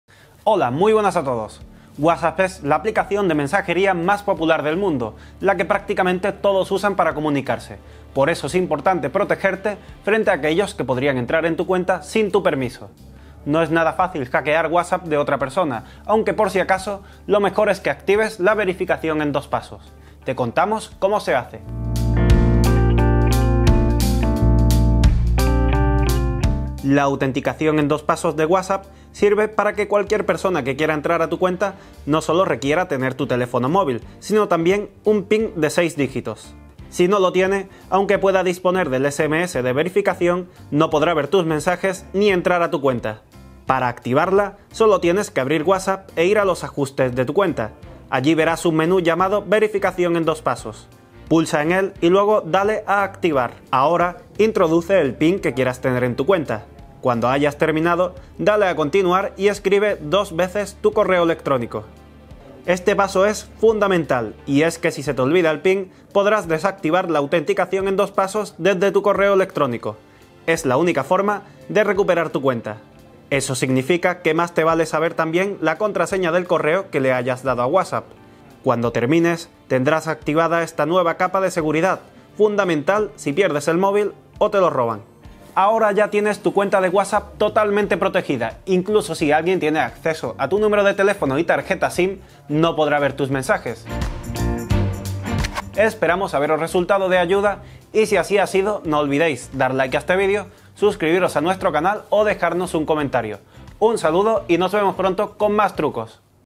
Haz tu WhatsApp más seguro con este truco
Si quieres proteger tu WhatsApp para evitar que accedan a tu cuenta, te contamos cómo puedes activar la verificación en dos pasos para que nadie pueda entrar incluso con acceso físico a tu teléfono.